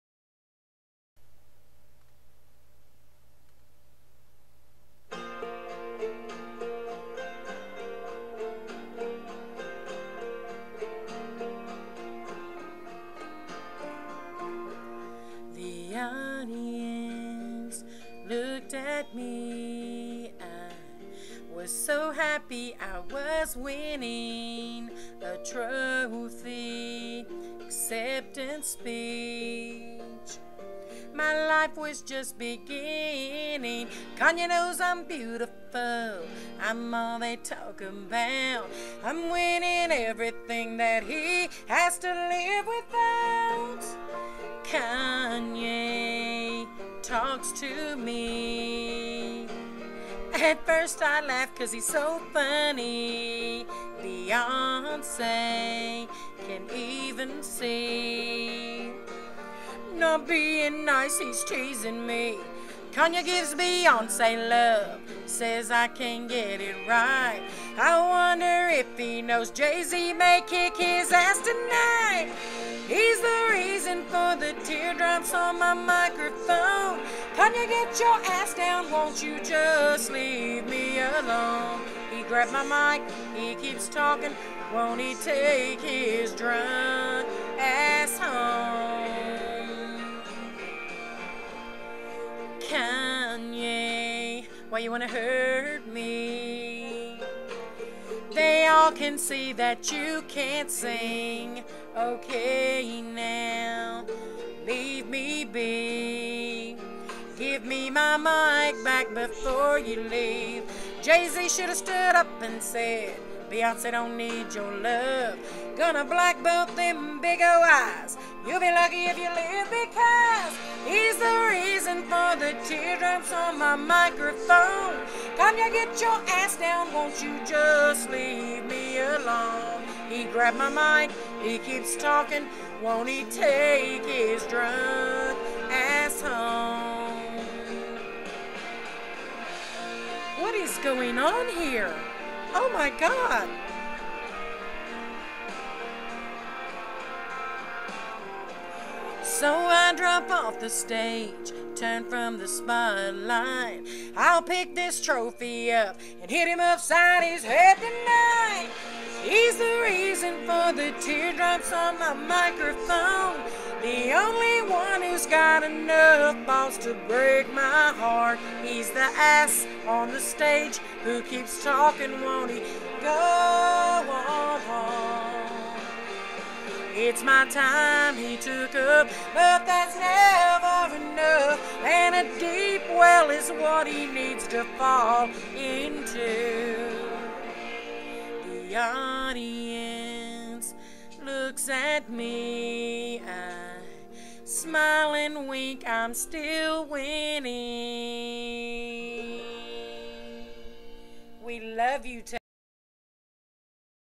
Taylor Swift's rebuttal towards Kanye
Taylor, you seem like too nice of a girl to make this clip... so i did it for u hon. Keep your chin up gal...hope this makes u laff!  We love your talent... keep doin'  what u do and God Bless!  Georgia